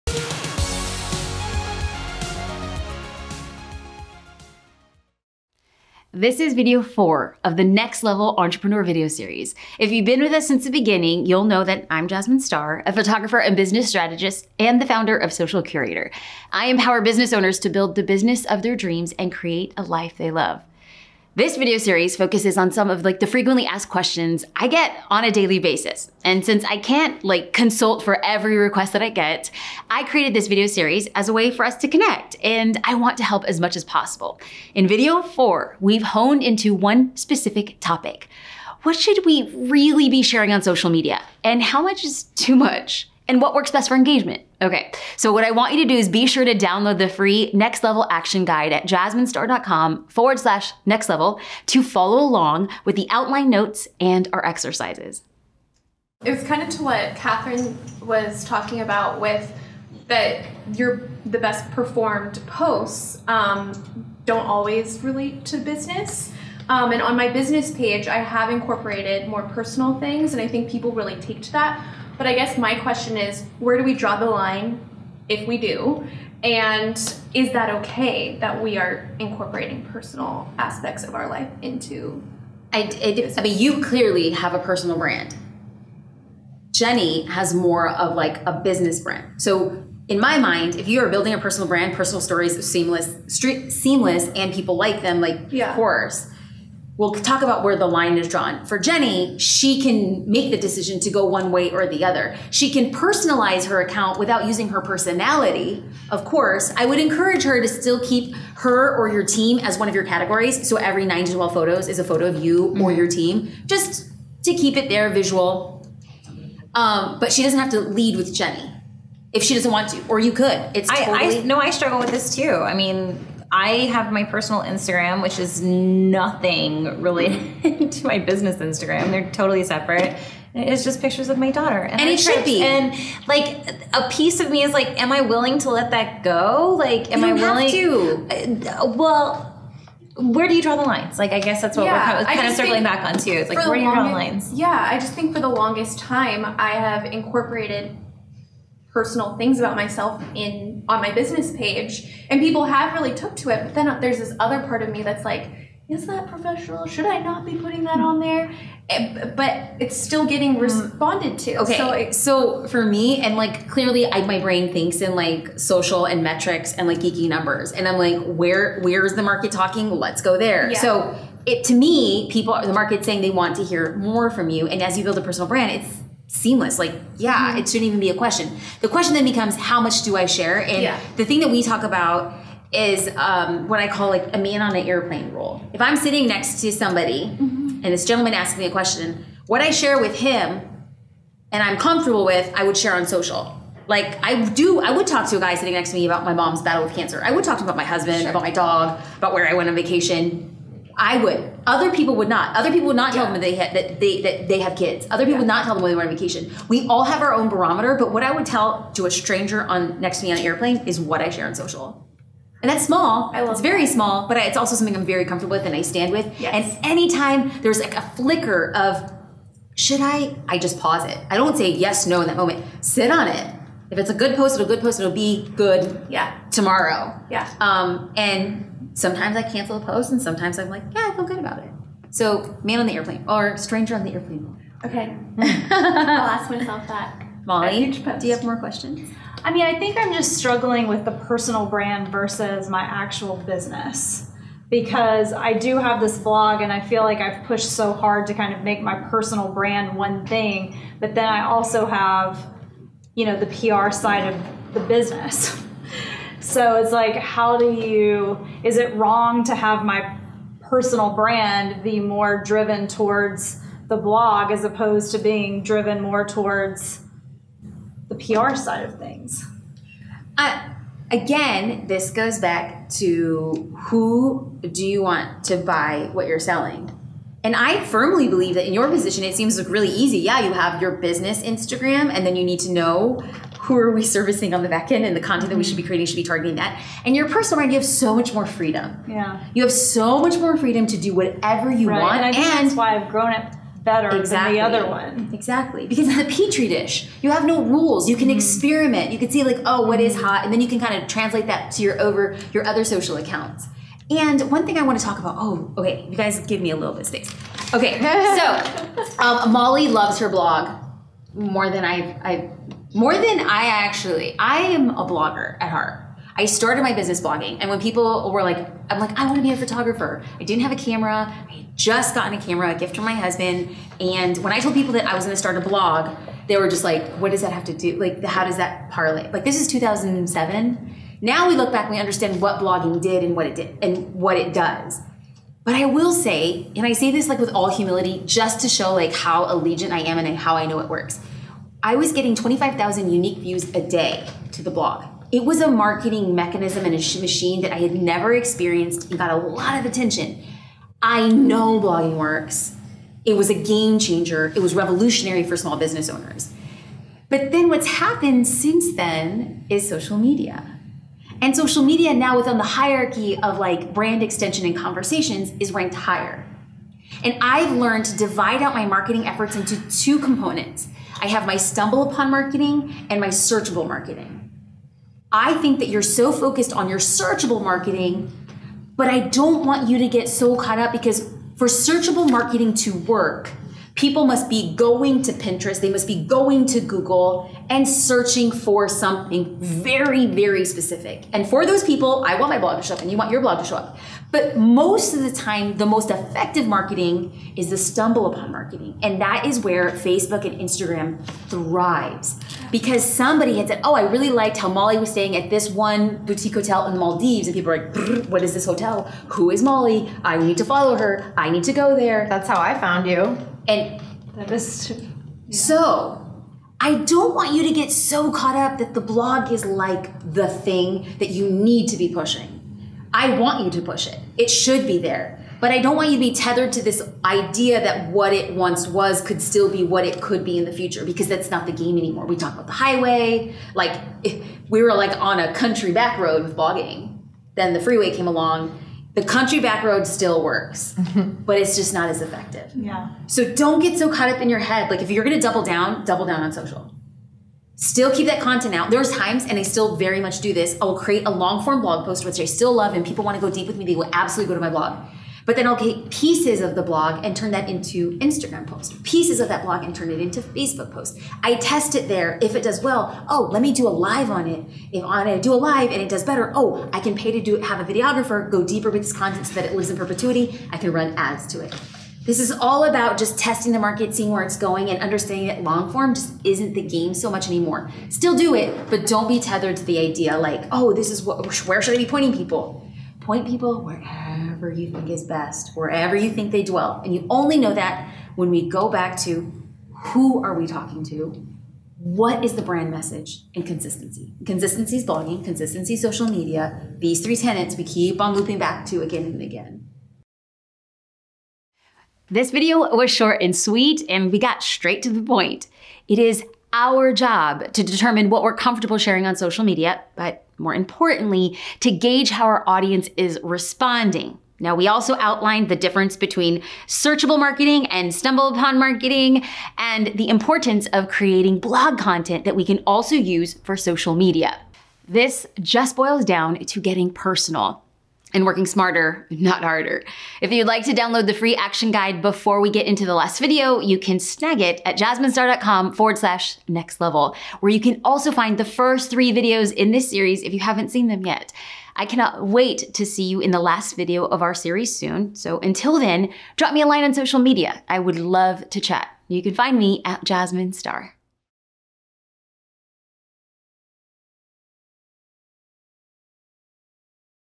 Next Level Entrepreneur : What is the best type of social media marketing?
Y'all this is video 4 of 5 of the Next Level Entrepreneur, a FREE series to take your business to...the next level ;) In this video, I chat with the entrepreneurs and explain what's the best type of social media marketing.
We chat about relevance (how much is too much when it comes to sharing?), how to get your business discovered on social media, and how to test your marketing efforts to know what your audience actually wants to see.
Be sure to download the free Action Guide book to ensure you're applying everything you learned today!